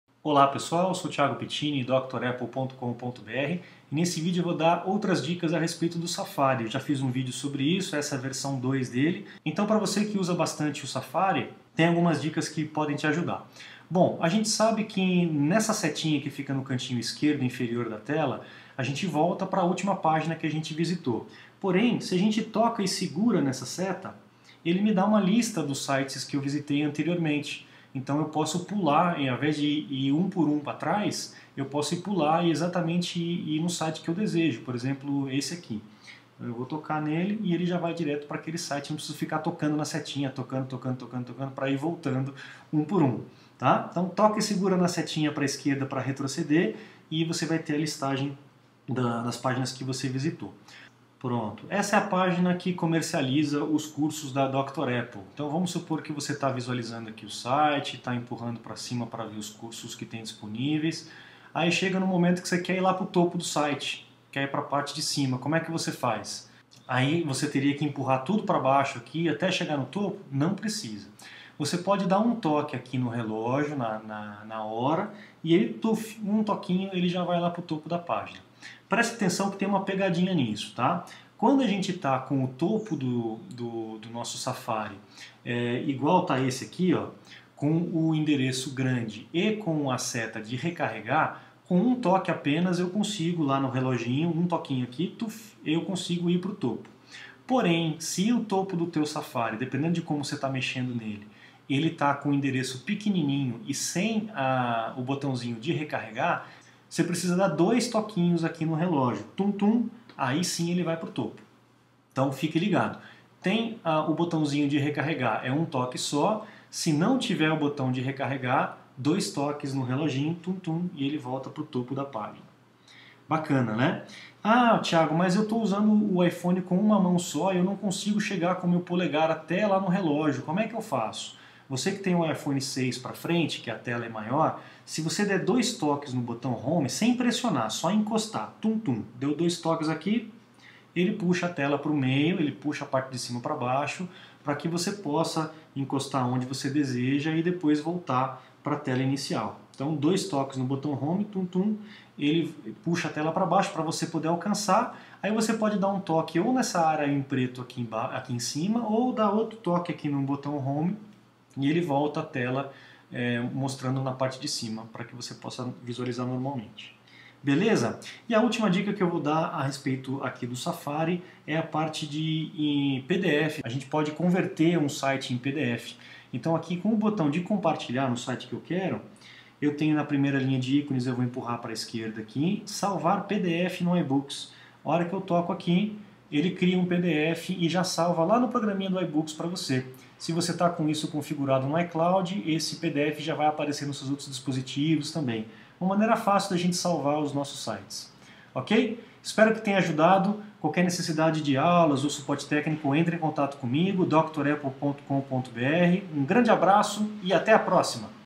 Aula iPhone e iPad - Dicas Safari 2
| Melhore sua navegação com dicas de como voltar às páginas anteriores, voltar ao topo do site, puxar a tela para baixo e salvar pdf usando o Safari. iOS9

Cursos, Aulas, Treinamento e Suporte em Mac, iPod, iPad, iPhone e toda linha Apple.